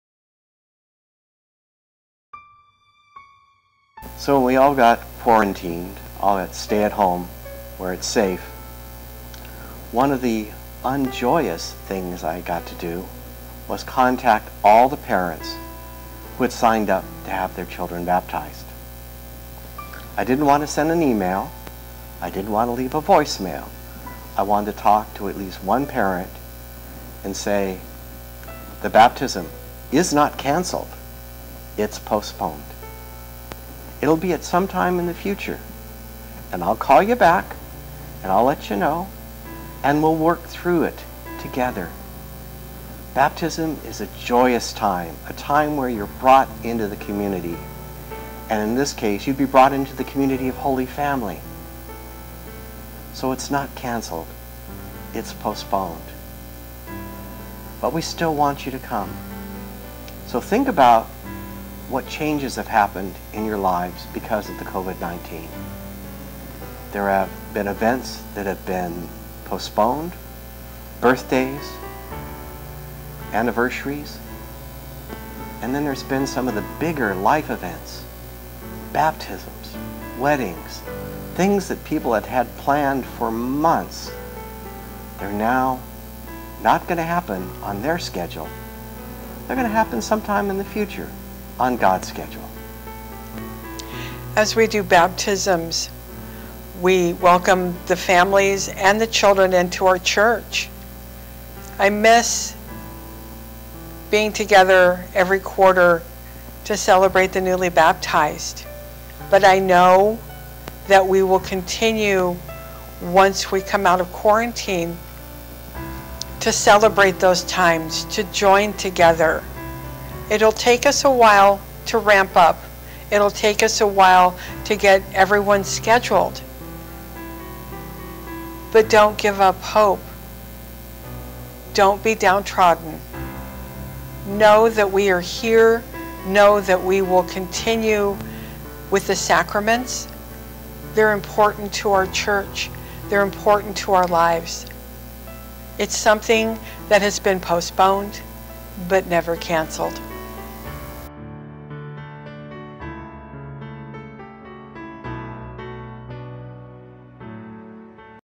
Prayer amid the Crisis, April 21, 2020
Holy Family Church is sharing inspiration and comfort throughout the health crisis.